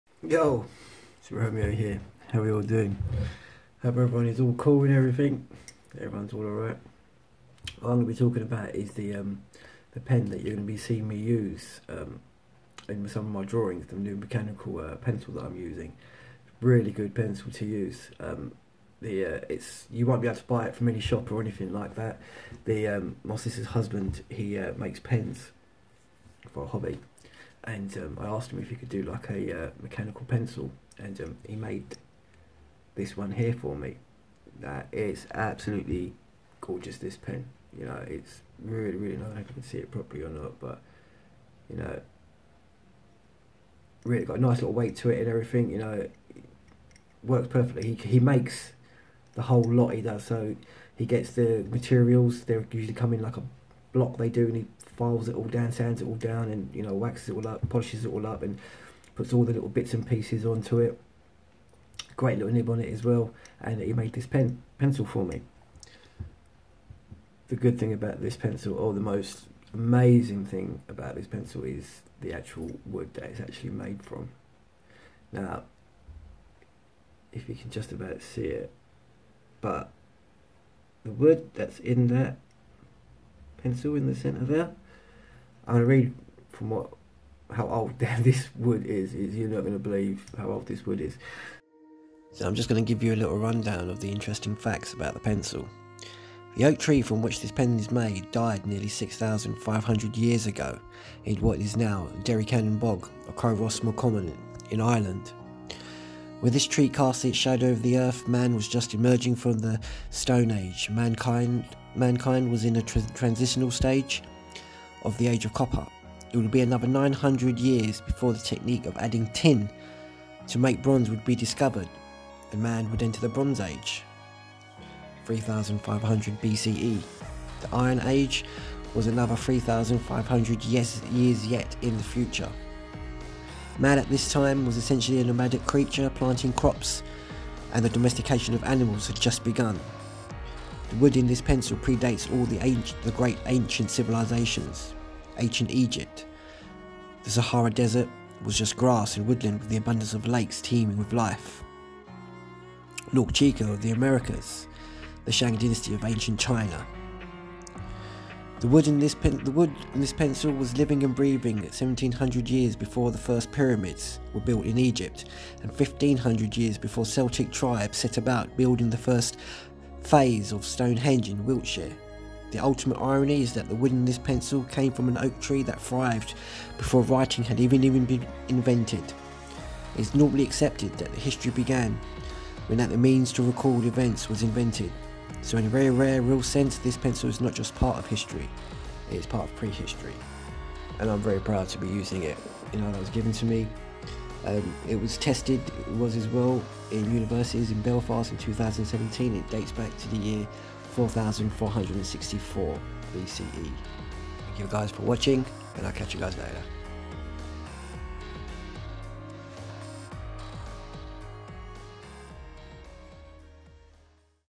My Historic Mechanical Pencil. Dating to the year 4464 BCE !!!
Unfortunately you wont be able to buy this pencil from any shop or anywhere , its one of a kind!!

History..

The Oak tree from which this pen is made died nearly 6500 yeras ago in what is now Derrycanan Bog of Co Roscommon in Ireland.

Man was just emerging from the Neolithic Stone Age. Man was in its transitional stage , The Chalcolithic era, or the Age of Copper. It would be another 900 years before the technique of adding Tin to make Bronze would be discovered and would enter the Bronze Age (3500 BCE). The Iron Age was 3500 years in the future!.

Man at this time was essentially a nomadic creature. Planting crops and domestication of animals had just begun. The wood in this pencil predates all the great  ancient civilization ; Ancient Egypt ( The Sahara desert  was grass and woodland with an abundance of lakes teaming with life), Norte Chico of the Americas, The Shang Dynasty of Ancient China, the Indus Valley Civilization and the oldest of them all the Sumerians of Mesopotamia now Southern Iraq where the earliest form of writing was first practiced .

The wood in this pencil was living and breathing 1700 years before the first pyramids were built in Egypt and 1500 years before Celtic Tribes set about building the first phase of Stonehenge in Wiltshire.

The Ultimate irony is that the wood in this pencil came from an Oak tree that thrived before writing had been invented. It is normally accepted that history began when the means to record events was invented. So in very sense, this pencil is not just part of history ; its part pf pre- history .

A sample of the tree from which this pencil has been made was submitted to the Dendrochronological Laboratory at Queens University Belfast in March 2017 for dendrochronological analysis. The tree was taken from Derrycanan Bog in County Roscommon Ireland , The dating was conducted by Mr. David M. Brown of the school of Natural and Built Environment, Archaeology & Palaeoecology , Belfast.
The analysis produced an estimate  for the death of the tree of 4464 BCE +/- 9 years .